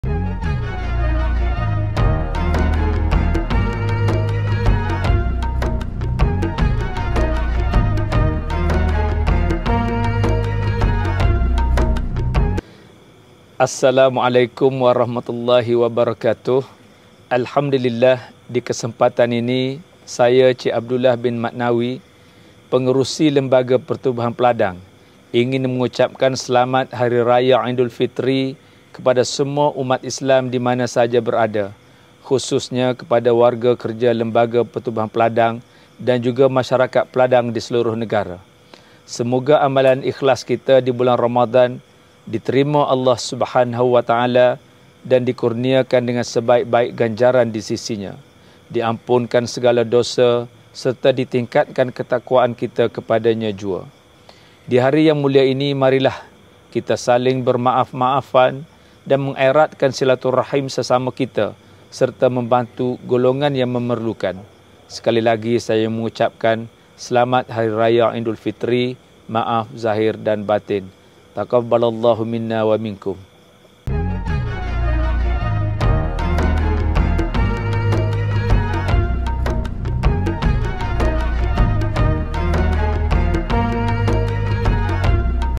UCAPAN SELAMAT HARI RAYA OLEH PENGERUSI LPP, YBHG. DATO’ HAJI CHE ABDULLAH BIN MAT NAWI
Di hari yang mulia ini marilah kita saling bermaaf-maafan dan mengeratkan silaturahim sesama kita serta membantu golongan yang memerlukan

Ikhlas daripada
YBhg. Dato' Haji Che Abdullah bin Mat Nawi

Pengerusi
Lembaga Pertubuhan Peladang